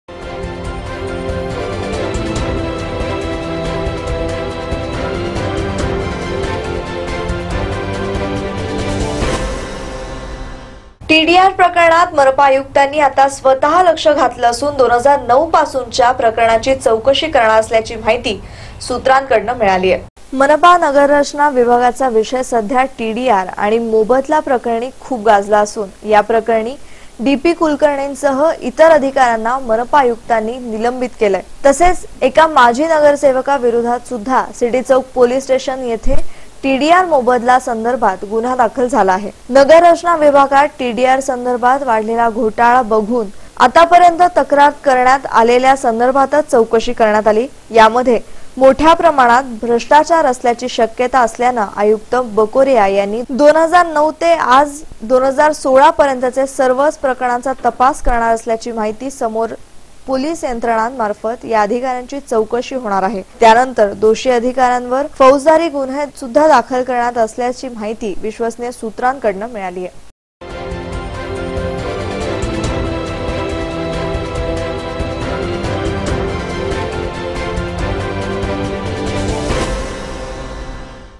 टीडीआर प्रकरणात मनपा आयुक्तांनी आता स्वतः लक्ष घातले असून २००९ पासूनच्या प्रकरणांची चौकशी करणार असल्
टीडीआर प्रकरणात मनपा आयुक्तांनी आता स्वतः लक्ष घातले असून २००९ पासूनच्या प्रकरणांची चौकशी करणार असल्याची माहिती सूत्रांनी दिली.